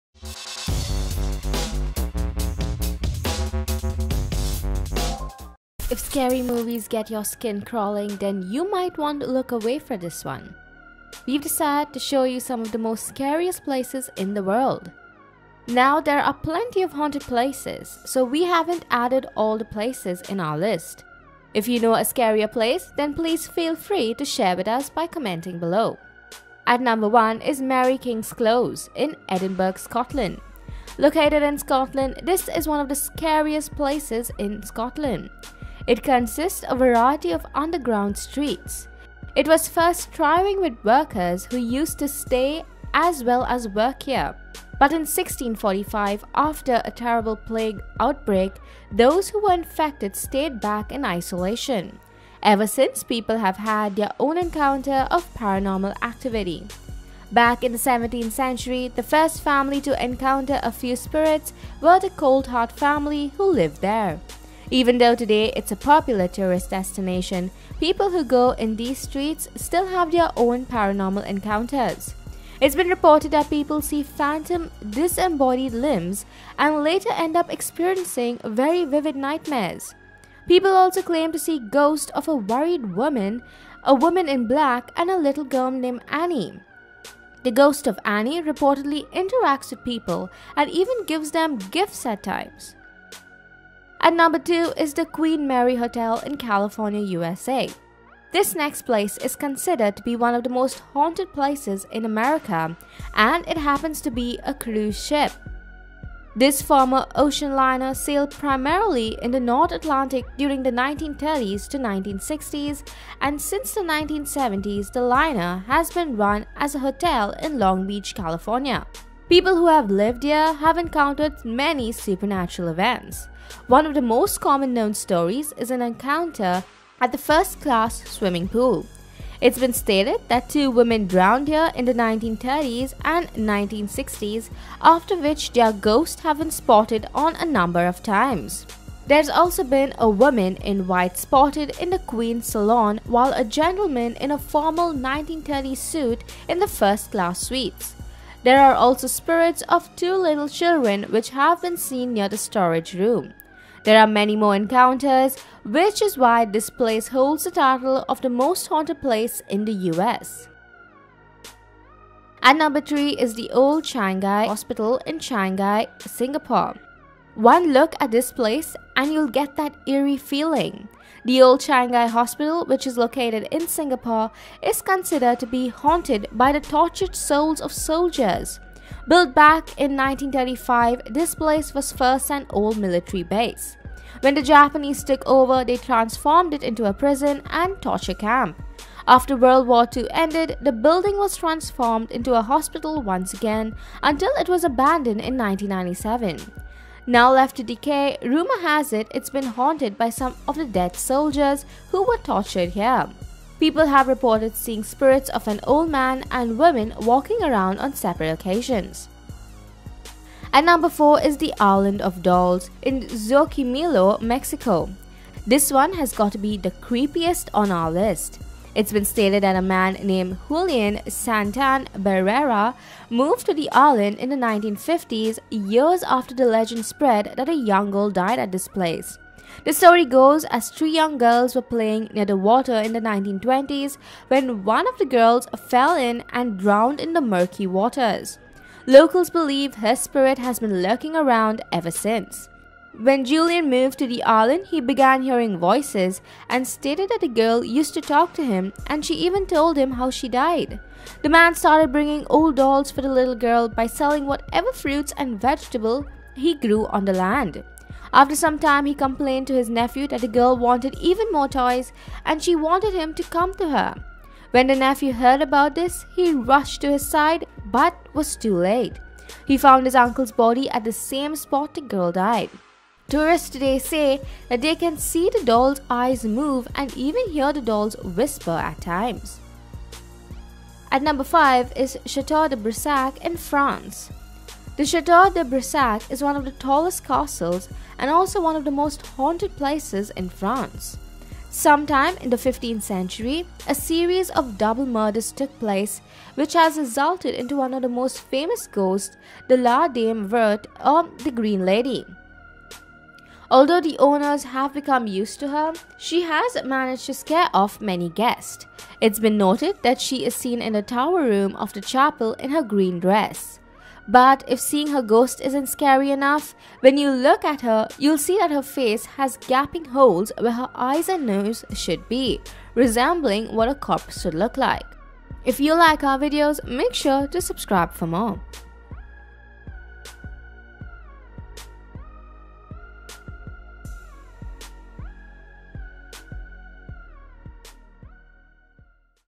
Top 5 Scariest Places in The World
If scary movies get your skin crawling then you might want to look away for this one. We’ve decided to show you some of the most scariest places in the world. Now there are plenty of haunted places so we haven’t added all the places.
Mary King’s Close – Edinburgh, Scotland
Located in Scotland, is one of the scariest places in Scotland. In consists a variety of underground streets. It was first a thriving with workers who used to stay as well as work here. But in 1645 after a terrible plague outbreak those who were infected stayed back in isolation. Ever since people have had their own encounter of paranormal activity. Back in the 17th century the first family to encounter a few spirits were The Coltheart family who lived there. Even though today it’s a popular tourist destination people who go in these streets still have their own paranormal encounters. It’s been reported that people see phantom disembodied limbs, and later end up experiencing very vivid nightmares. People also claim to see the ghost of a ‘worried woman’, a woman in black, and a little girl named Annie. The ghost of Annie reportedly interacts with people and even gives them gifts at times.
 The Queen Mary Hotel – California, USA
This next place is considered to be one of the most haunted places in America and it happens to be a cruise ship. This former ocean liner sailed primarily in the North Atlantic during the 1930’s to 1960’s and since the 1970’s the liner has been run as a hotel in Long Beach, California. People who have lived here have encountered many supernatural events. One of the most common known story is an encounter at from the First Class Swimming Pool. It’s been stated that two women drowned here in the 1930’s and 1060’s after which their ghost have been spotted on a number of times. There’s also been a woman in white spotted spotted in the Queen’s Salon while a gentleman in a formal 1930’s suit in the First Class suites. There are also spirits in two little children have been seen near the storage room. There are many more countless encounters which is why this place holds the title of most haunted place in the U.S.
Old Changi Hospital – Changi, Singapore
One look at this place and you’ll get that eerie feeling. The old Changi hospital which is located in Singapore is considered to be haunted by the tortured souls of soldiers. Build back in 1935, this place was first an old military base. When the Japanese took over they transformed it into a prison and torture camp. After World War II ended, the building was transformed into a hospital once again until it was abandoned in 1997. Now left to decay, rumour has it that it’s being haunted by some of the dead soldiers who were tortured here. People have reported seeing apparitions of an old man and woman walking around on separate occasions.
Island of the Dolls – Xochimilco, Mexico
The next haunted place on our list is in Mexico. The island of dolls. This one has got to be the creepiest on our list. It’s been stated that a man named Julian Santan Barrera moved to the island in the 1950’s years after the legend spread that a young girl died at the place. The story goes as three young girls were playing near the water in the 1920’s, when one of the girls fell in and drowned in the murky waters. Locals believe her spirit has been lurking around ever since. When Julian moved to the island he began hearing voices and stated that the girl used to talk to him and she even told him how she died. The man started bringing old dolls for the little girl by selling whatever fruits and vegetable he grew on the land. After some time he complained to his nephew that the girl wanted even more toys and that she wanted him to come to her. When the nephew heard about this he rushed to his side but was too late. He found his uncle's body at the same spoy the girl died. Tourist today say that they can see the dolls eyes move and even hear the dolls whisper at time.
Château de Brissac, Maine-et-Loire, France
The Chateau de Brissac, is one of the tallest castles and also one of the most haunted places in France.  Sometime in the 15th century a series of double murders took place which has resulted into one of the most famous ghost, the la Dame Verte, or “Green Lady”. Although the owners have become used to her, she has managed to scare off many guests. It is been noted that she’s seen in the tower room of the chapel in her green dress. But if seeing her ghost isn not scray enough, when you look at her you’ll see that her face has gaping holes were her eyes and nose should be, resembling what a corpse would look like.